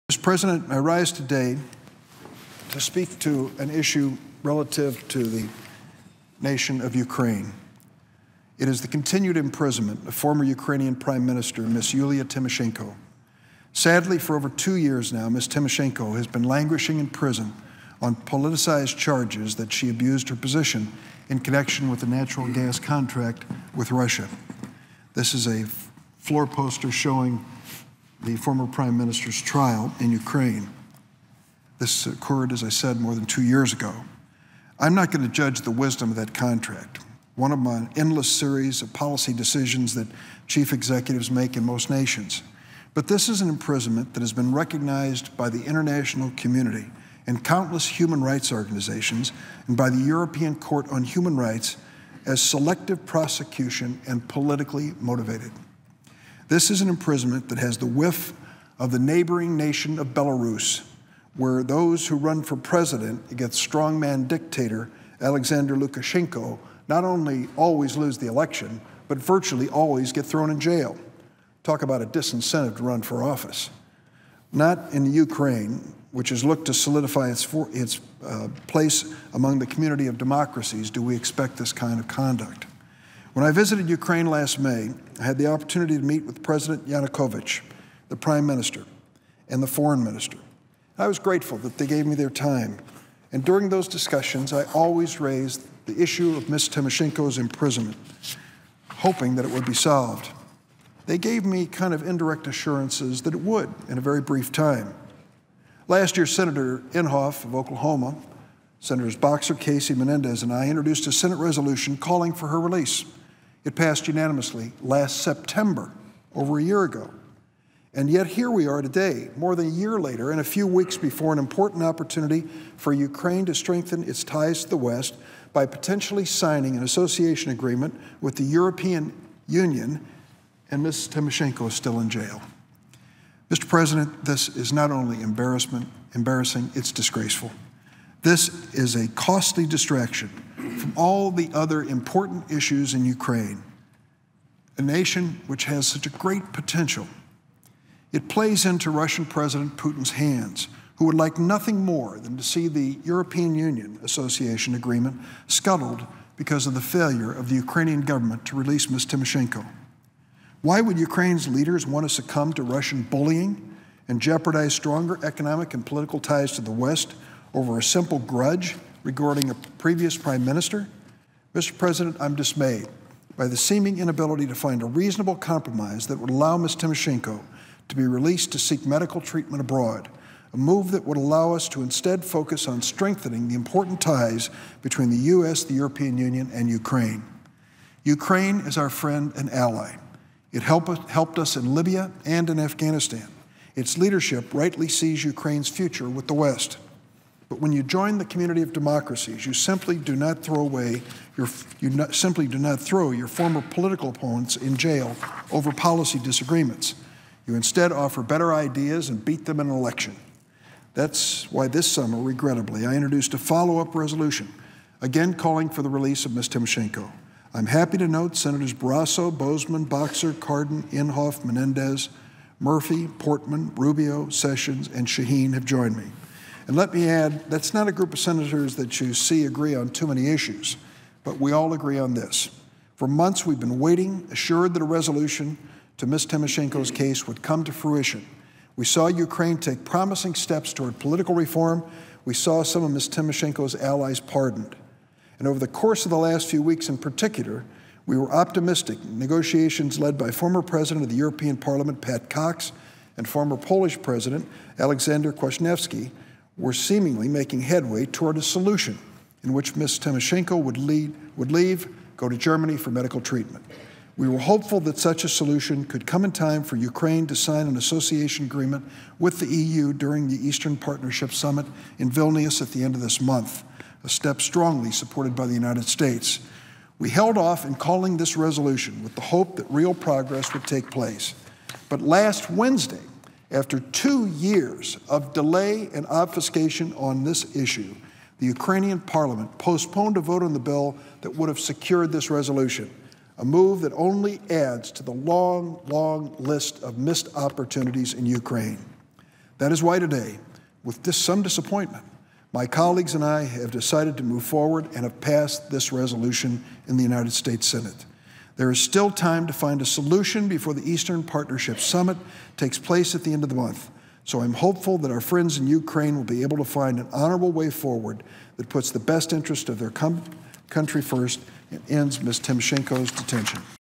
Calling for the Release of Former Ukraine PM Yulia Tymoshenko
Assistant Senate Majority Leader and member of the Senate Foreign Relations Committee, Dick Durbin (D-IL), today led the unanimous passage of a Senate resolution calling for the release of Ukraine's former Prime Minister, Yulia Tymoshenko.